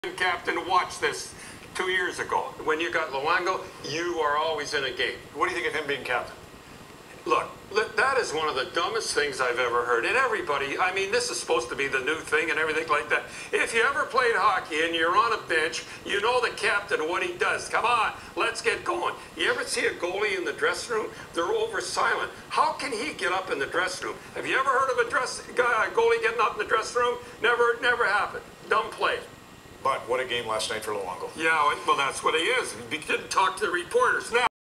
Don Cherry's thoughts on Roberto Luongo's captaincy of the Vancouver Canucks posted by headtothe.net
Don Cherry's thoughts on Roberto Luongo's captaincy of the Vancouver Canucks